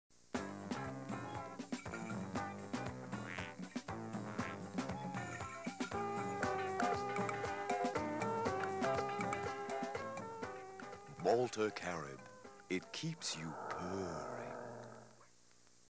Malta Carib Gally - BB&B
Everard "Gally" Cummings Plays for Malta Carib.
Directed by Tony Da Silva.
VA Crew Donald Lee Fook, Andy Smart, Mike Gonzales.
Editor - Tim